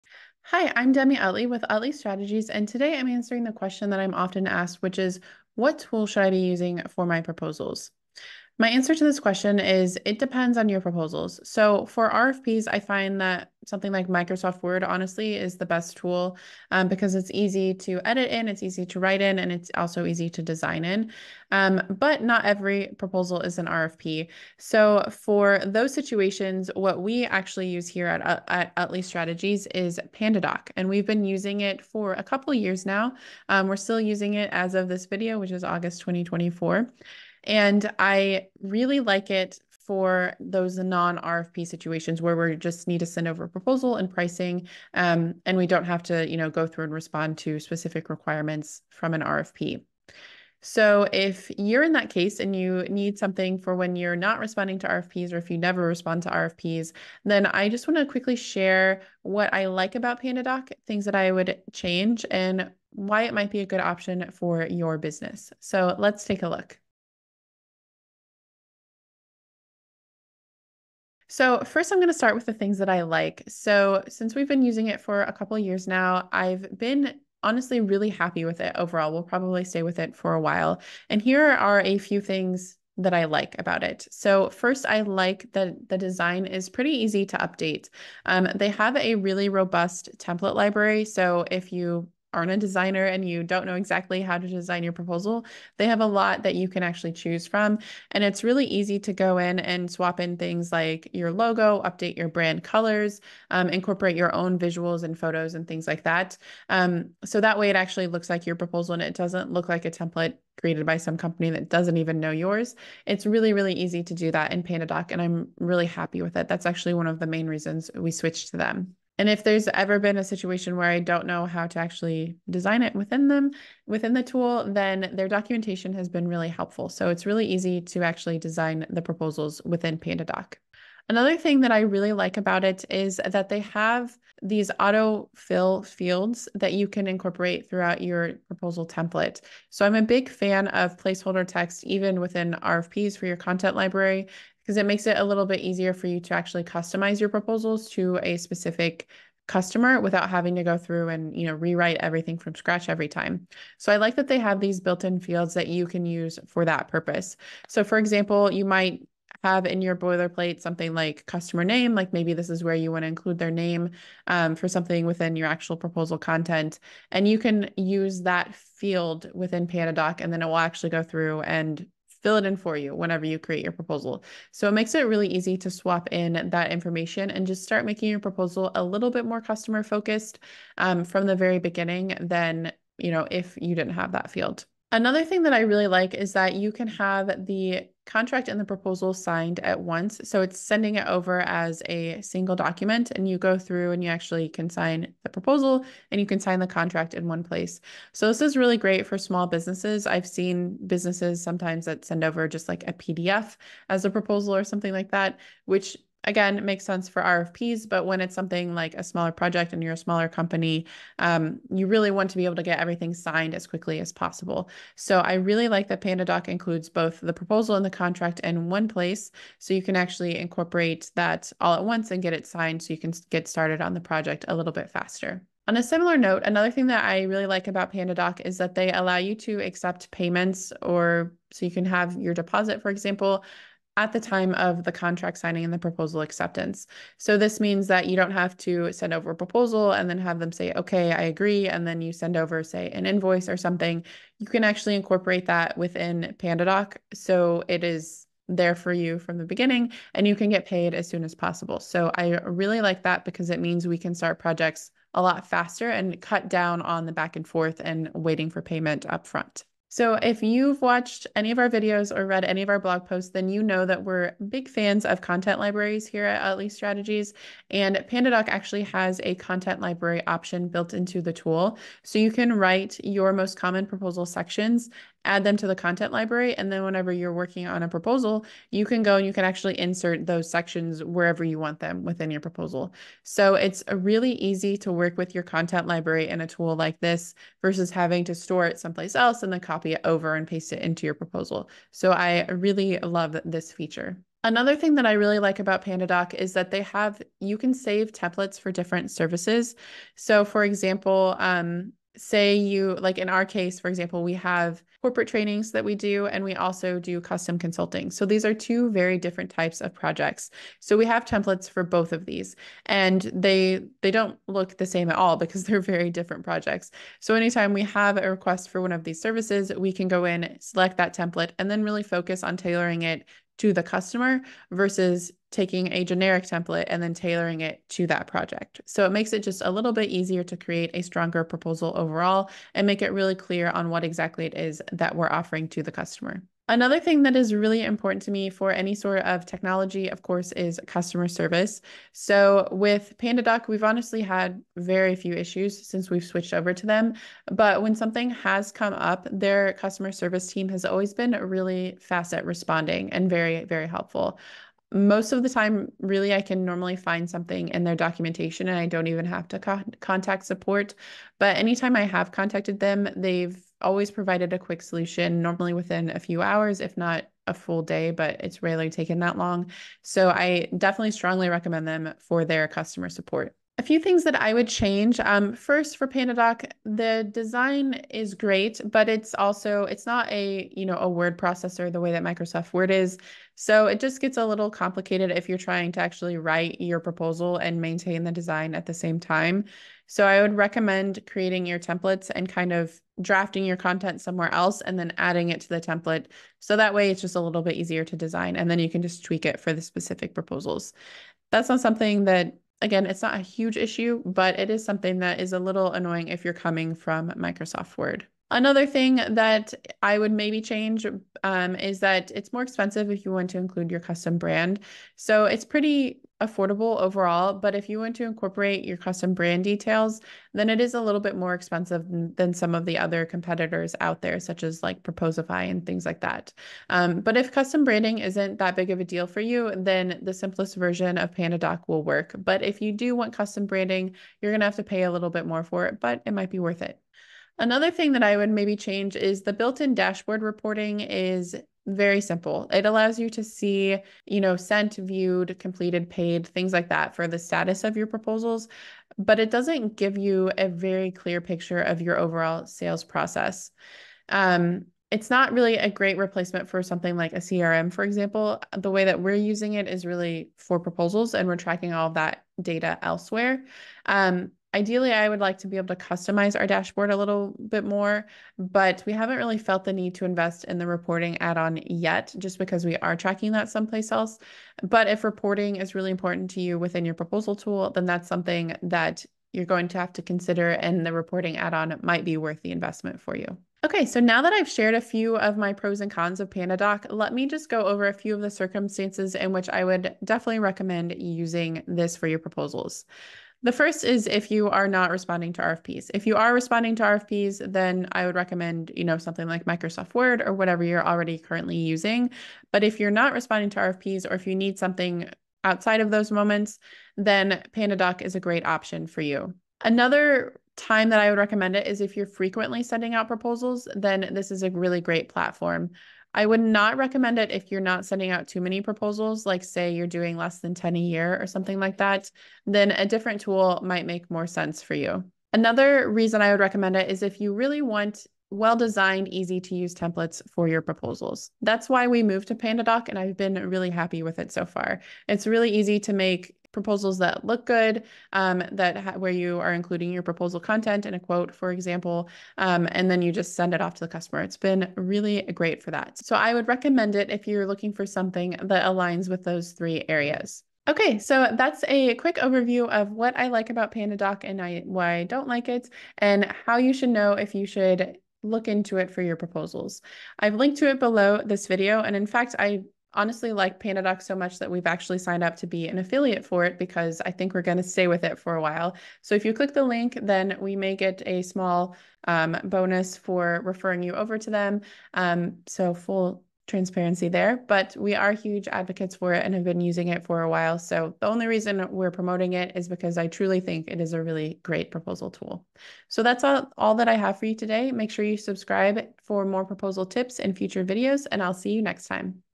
PandaDoc for Proposals: An Honest Review
Not sure what tool to use for your proposals?
In this video, I share what I like and don’t like about the proposal software, PandaDoc.

00:00:00 - Introduction
00:01:11 - What I like
00:01:25 - Easy to Update Design
00:02:11 - Autofill Fields
00:03:05 - Contract and Proposal Signed at Once
00:03:46 - Accept Payment At Contract Signing
00:04:20 - Content Library
00:04:54 - Templates for Different Services
00:05:36 - Responsive Customer Service
00:06:15 - What I’d Change
00:06:20 - Complicated to Write In
00:06:49 - More Expensive for Custom Branding
00:07:18 - Reporting
00:08:04 - Is PandaDoc right for you?
00:08:14 - Not Responding to RFPs
00:08:30 - Frequently sending proposals
00:08:46 - Want design and easy to use

🤩 PROPOSAL WRITING 🤩